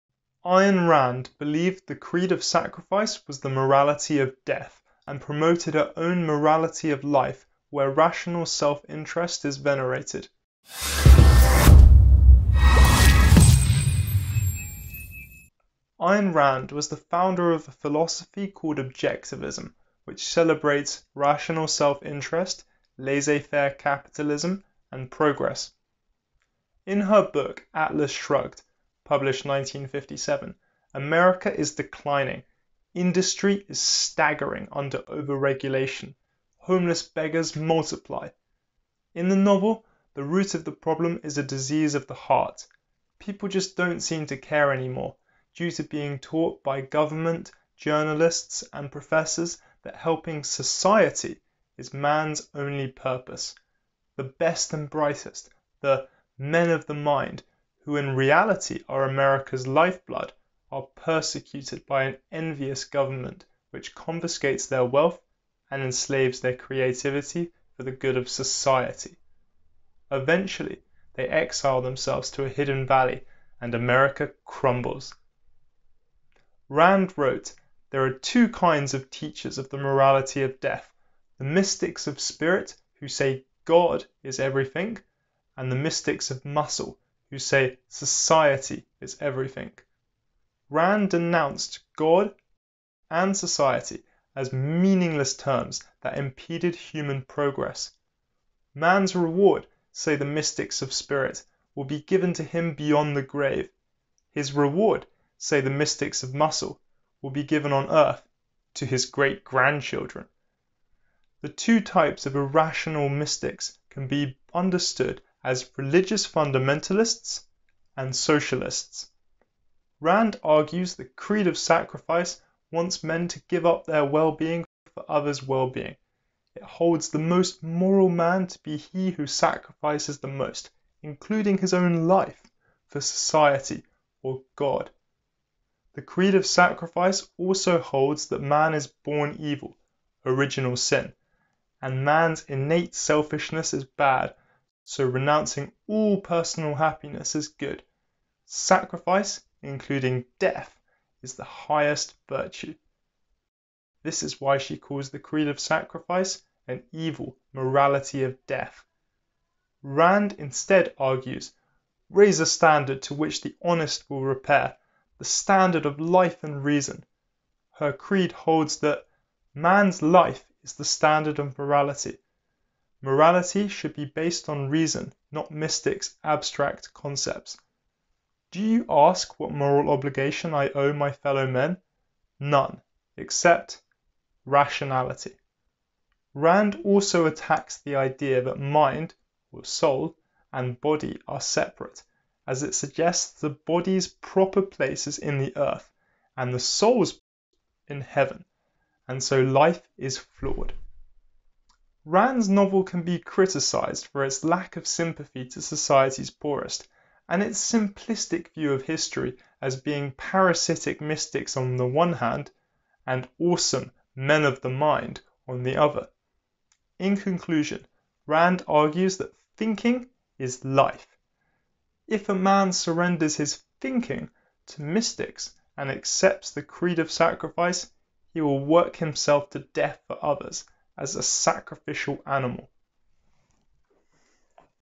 Ayn Rand on the Morality of Life
Ayn Rand, a Russian American novelist and philosopher (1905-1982) believed that the creed of Sacrifice is the morality of death. Conversely rational self-interest was to her the morality of life. She wrote these ideas in Atlas Shrugged (1957)  a massively popular novel with Objectivists, and neoliberals.

Sources: quotes from page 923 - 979, John Galt’s speech.